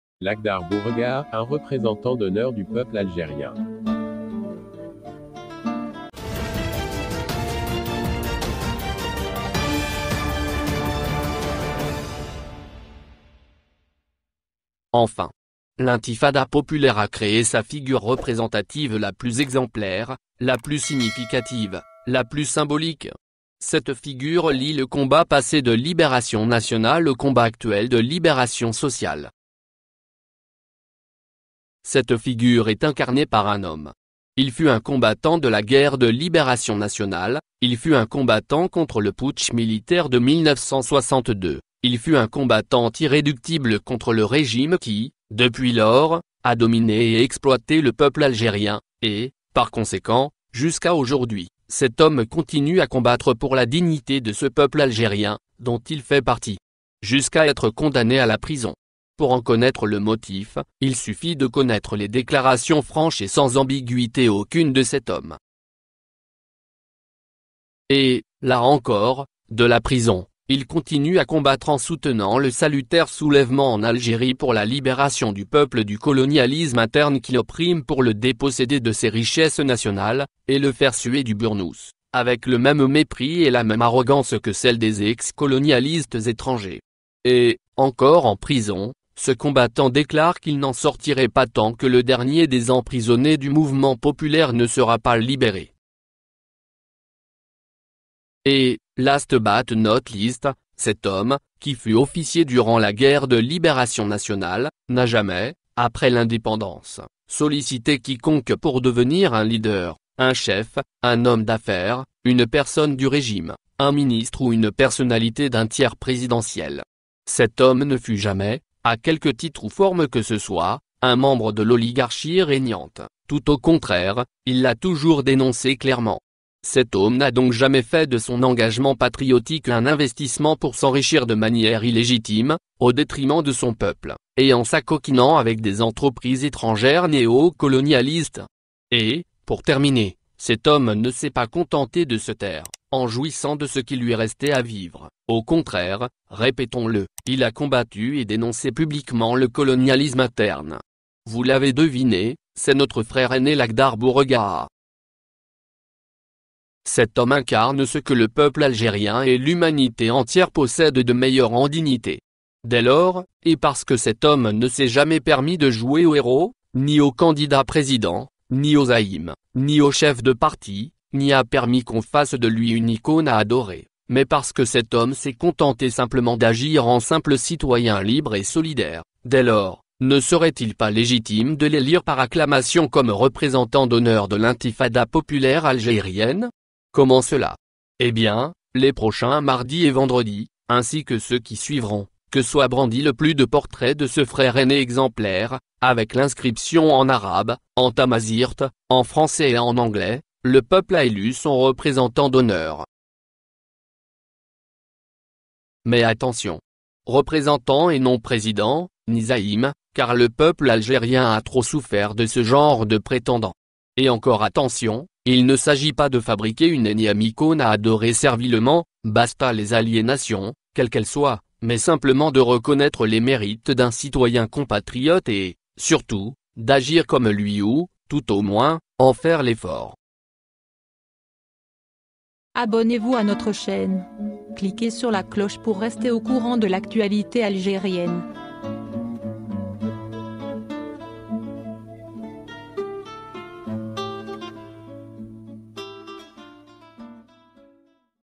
Algérie: Les algériens ont élu leur Représentant d’Honneur !
Lakhdar Bouregaa homme exceptionnel aimé par les algériens.